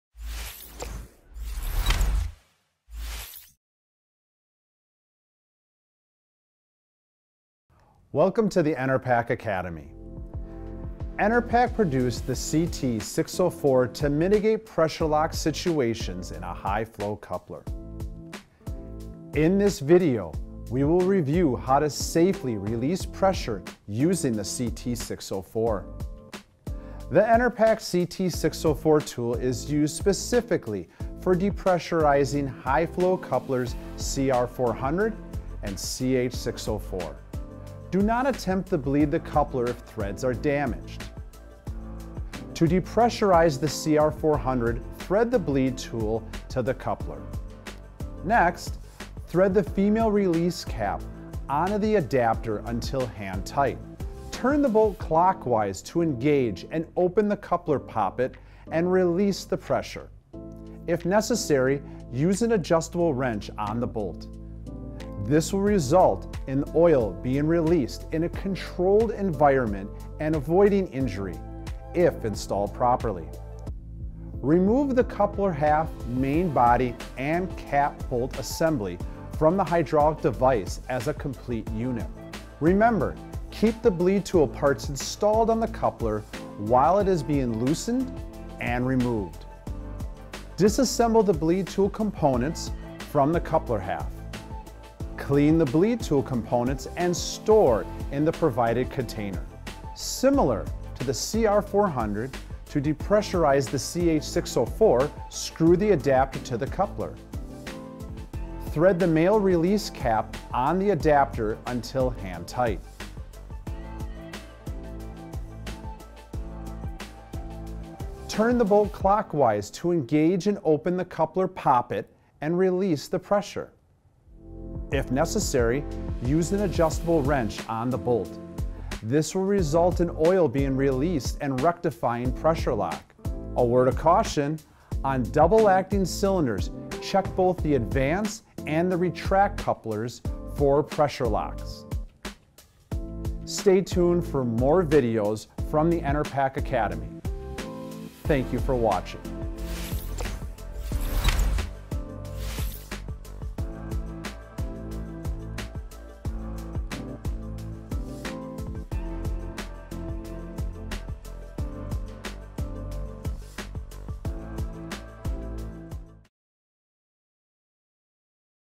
How To Depressurize Hydraulic Couplers and Safely Bleed a Coupler with the Enerpac CT604
Watch to learn how to depressurize hydraulic couplers.
The Enerpac CT604 tool is used specifically for depressurizing high-flow couplers CR400 and CH604. The CT604 relieves hydraulic back pressure by safely bleeding the hydraulic coupler. Minimize injuries from projectile parts and under-skin hydraulic fluid injections by eliminating unsafe coupler bleeding practices. The CT604 is Enerpac-engineering safe for use at 10,000 psi (700 bar).